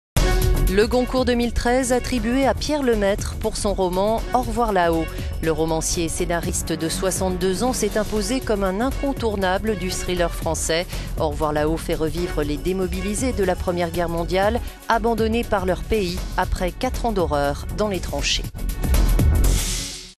France : le Goncourt attribué à Pierre Lemaitre (Au revoir là-haut)
Le Goncourt 2013 a été attribué à Pierre Lemaitre pour son roman "Au revoir...

Le Goncourt 2013 a été attribué à Pierre Lemaitre pour son roman "Au revoir là-haut". Le romancier et scénariste de 62 ans s'est imposé ces dernières années comme un incontournable du thriller français. "Au revoir là-haut" fait revivre les démobilisés de la Première guerre mondiale, abandonnés par leur pays après quatre ans d'horreur dans les tranchées.